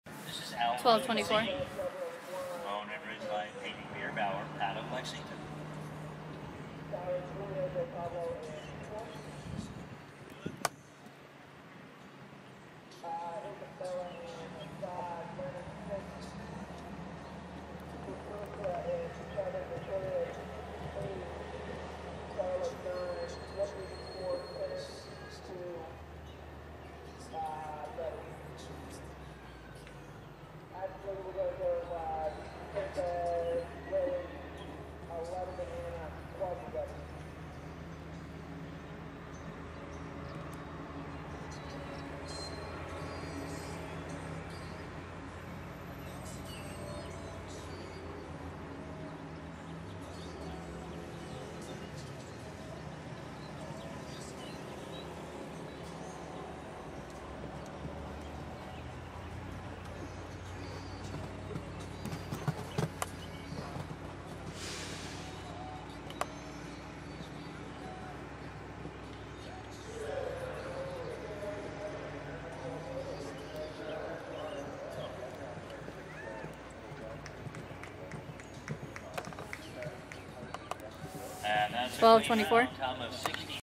Video of ALFIE Z ridden by PAYTON BEERBOWER from ShowNet!
ALFIE Z ridden by PAYTON BEERBOWER at Kentucky Spring Horse Show 2019.  This round is from class 460, LOW A-O JUMPER II, Sec 2.1 and finished 6 of 37.